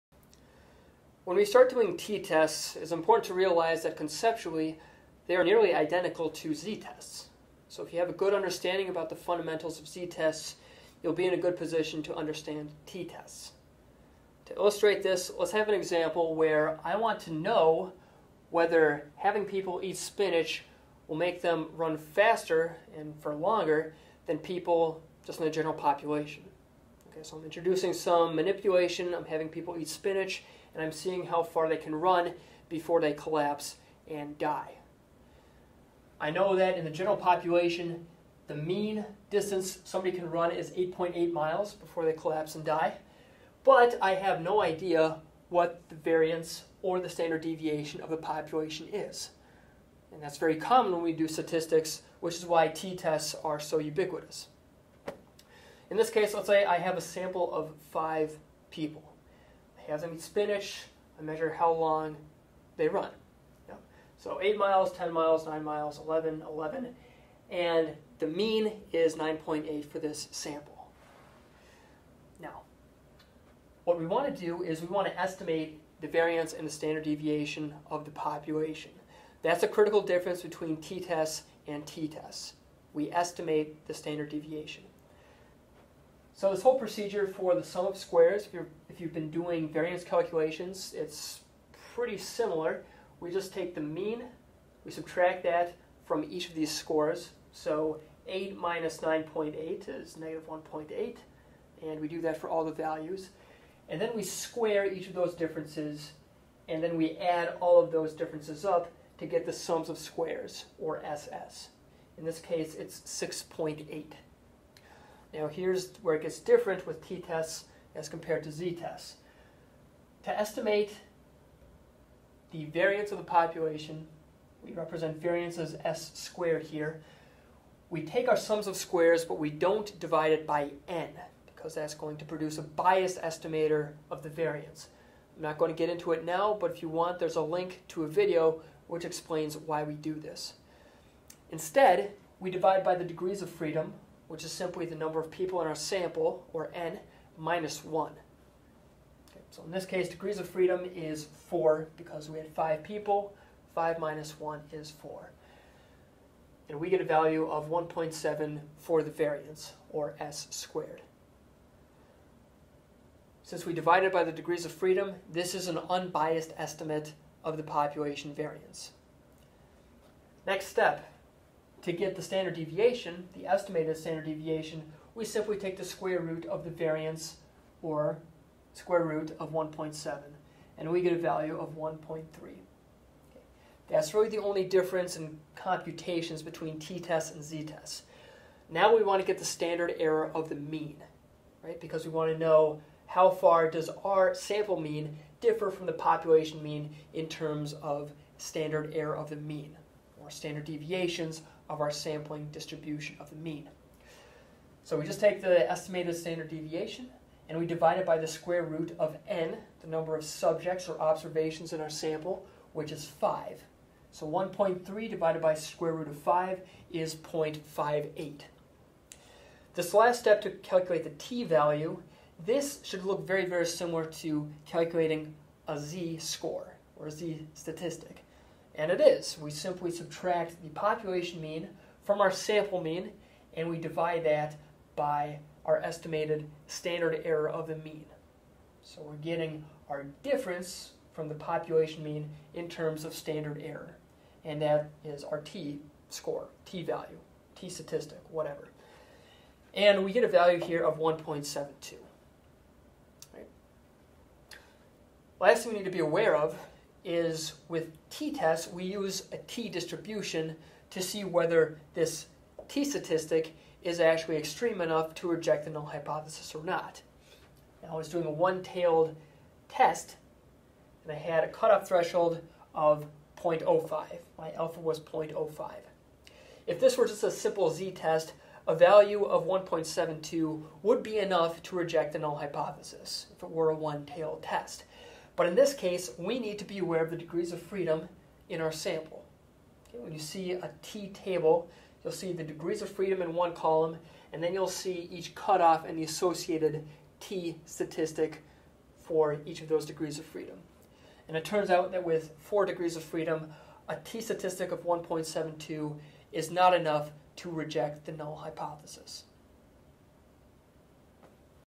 One-Sample T-Tests
Introduction to one-sample t-tests, and how they can change your life. Or not.